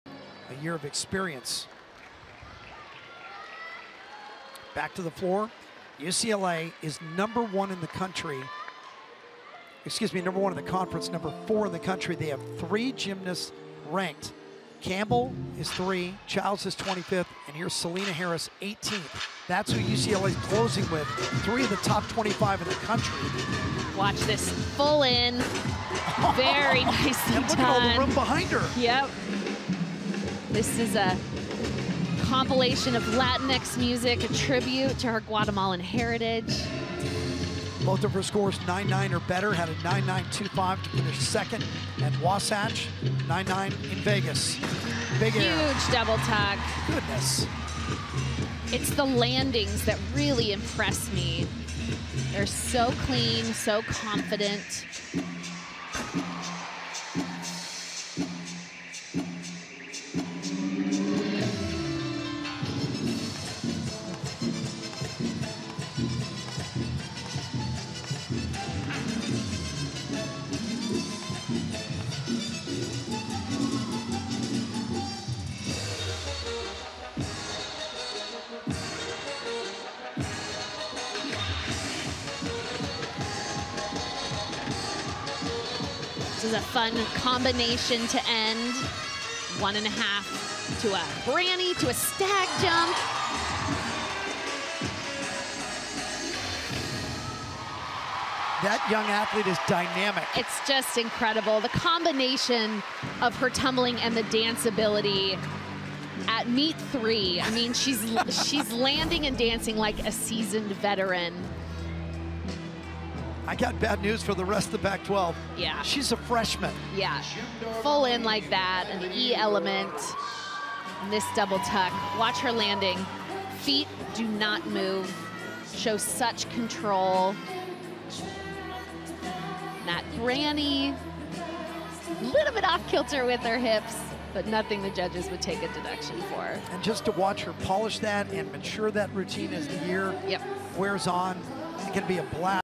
UCLA’s Selena Harris performs 9.925 floor routine at Washington | Pac-12 Gymnastics
UCLA gymnast Selena Harris shined with a 9.925 on floor in a meet at Washington on Jan. 21, 2023.